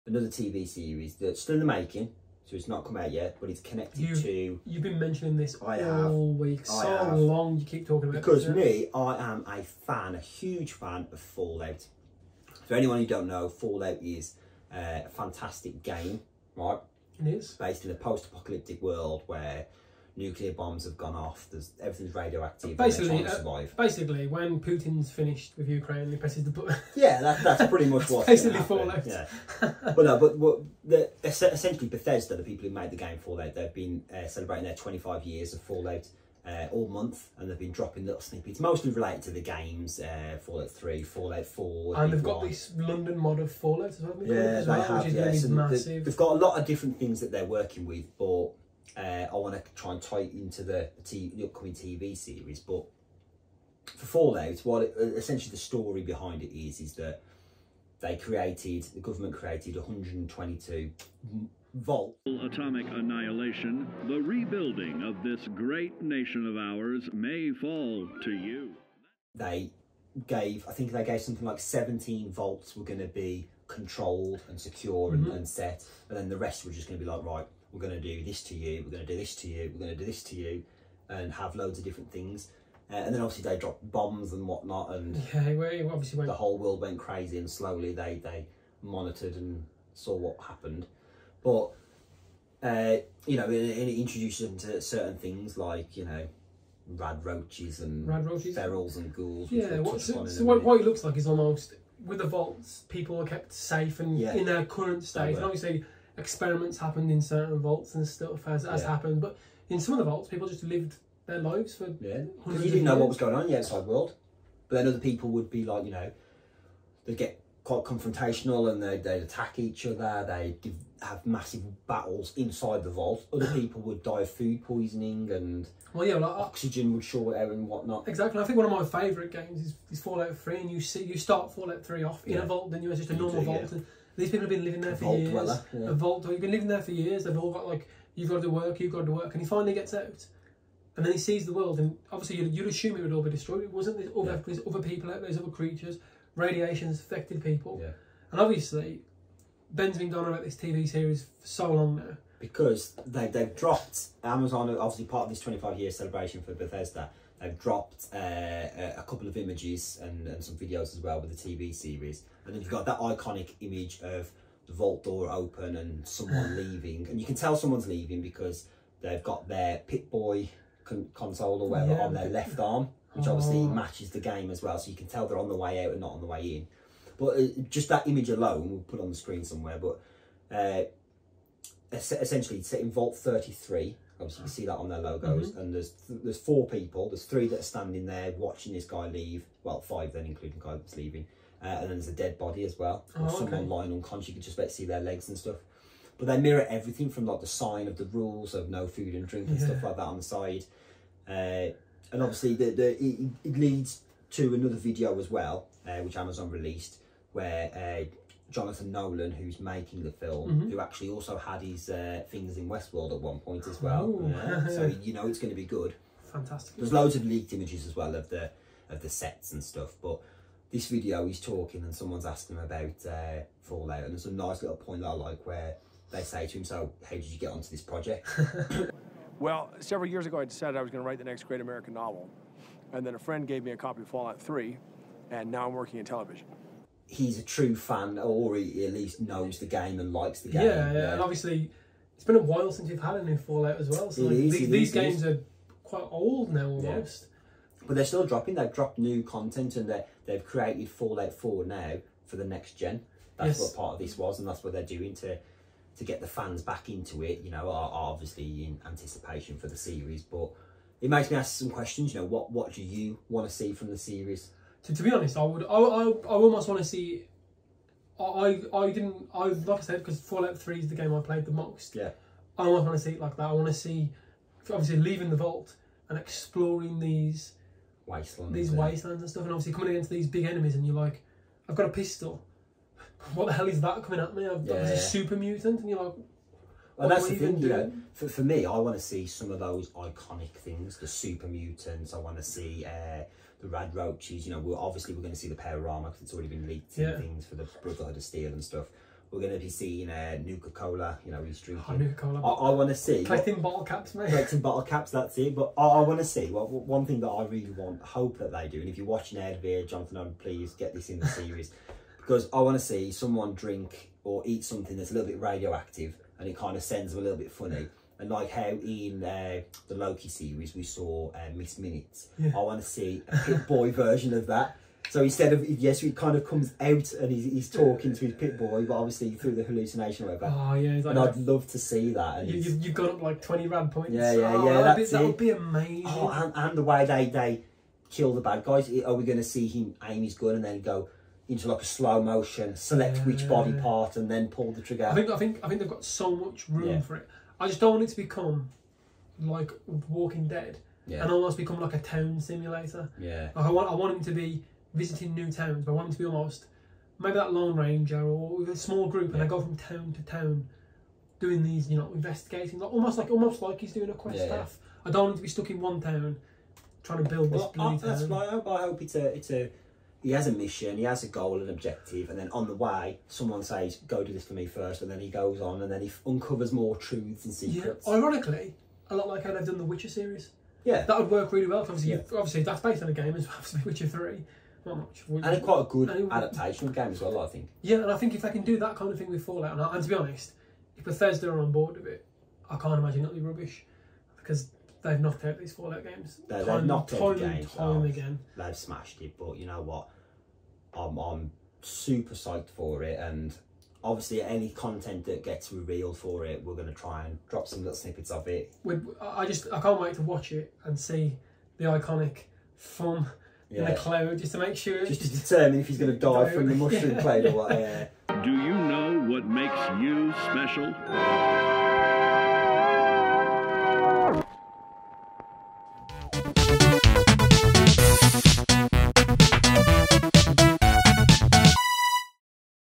Watch Out For That Radroach | Fallout Series Thoughts & Predictions
We're looking forward to the Fallout Tv Series